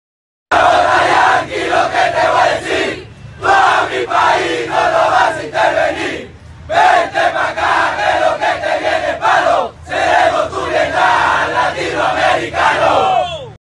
Venezuelan Soldiers to US Coup attempt ''We will be your Latin American Vietnam''
Venezuelan Soldiers response to OperacionLibertad, a US backed coup that could trigger a war.